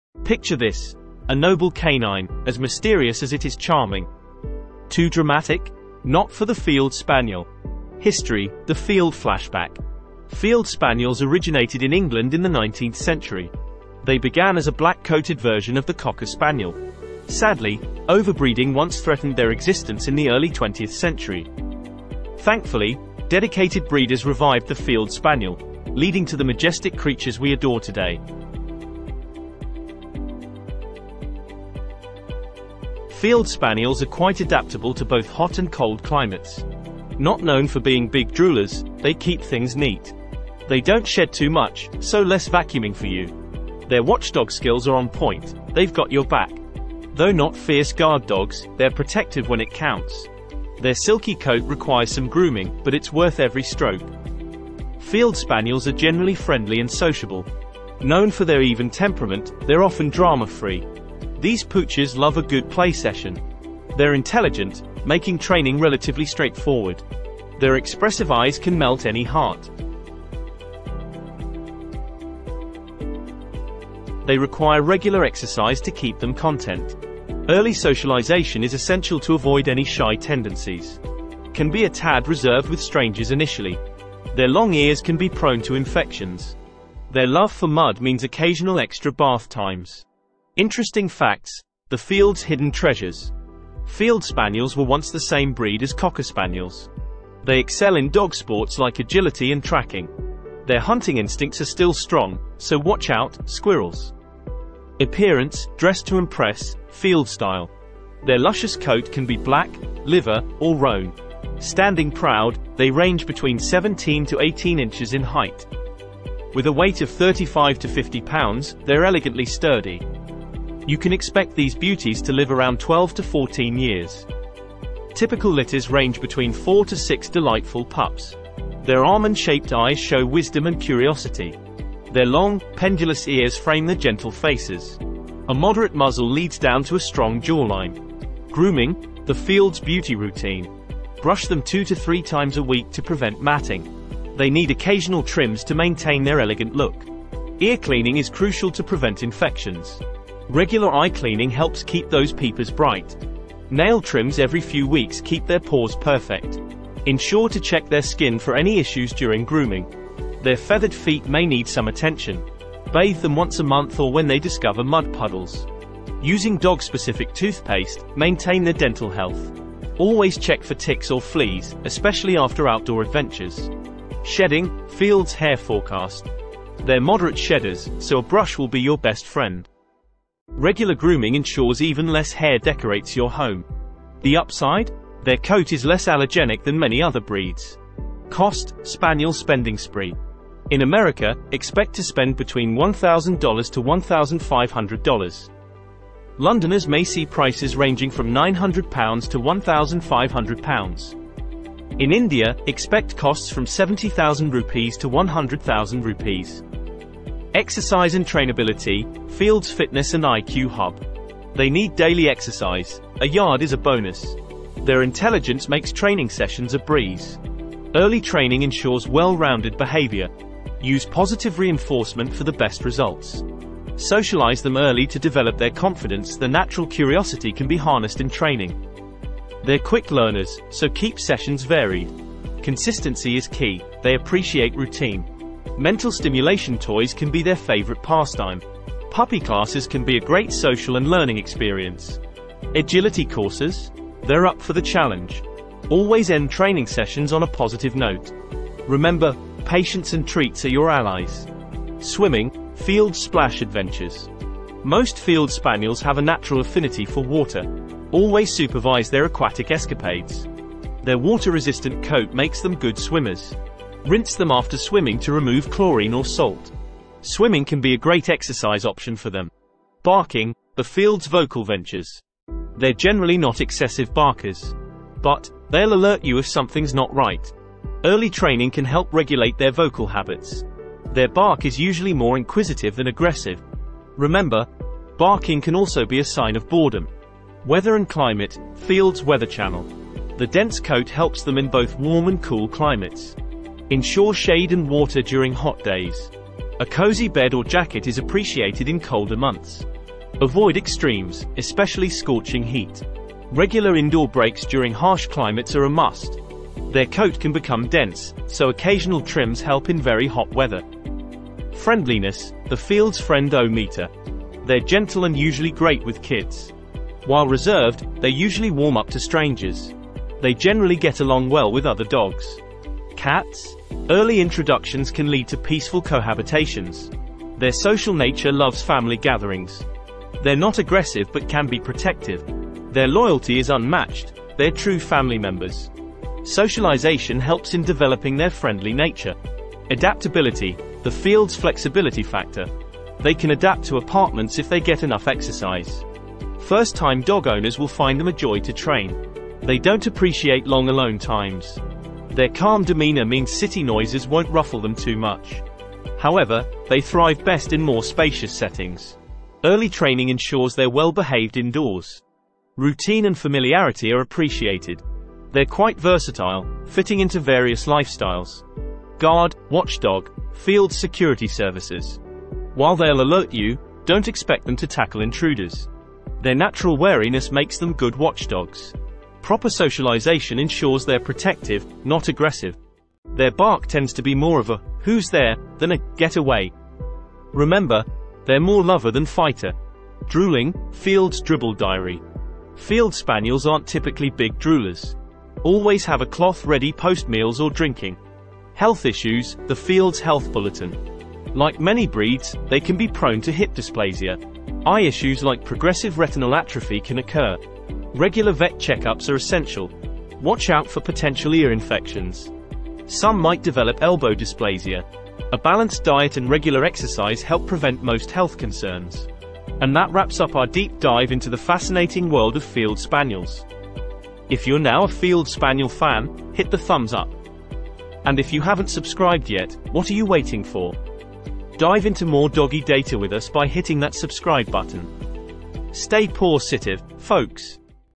Field Spaniel Top 10 Facts | Pros and Cons you must know
This video is all about field spaniel pros and cons